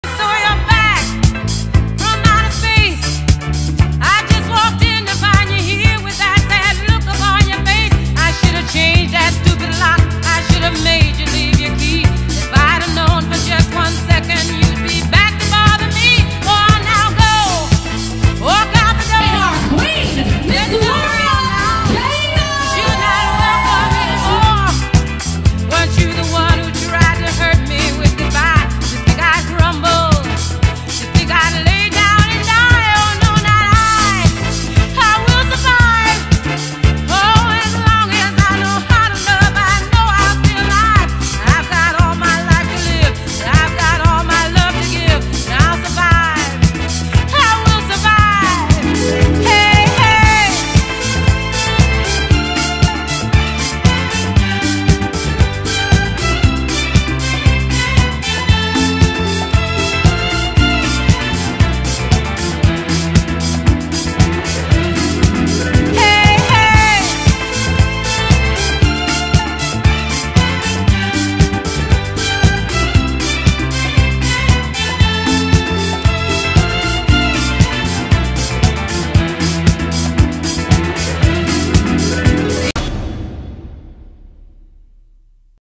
AMP DJ/BAND HYBRID | Gloria Gaynor Birthday Party NYC | New York Cruises
We are so honored to have been the entertainment for Gloria Gaynor's 80th Birthday celebration!!
Ms. Gaynor chose our 8 Piece DJ/BAND HYBRID, Electric Violin Cocktail Hour, and our Video Confessional Booth/Photo Booth!

We specialize in LIVE BAND/DJ HYBRIDs led by world-class vocalists and musicians!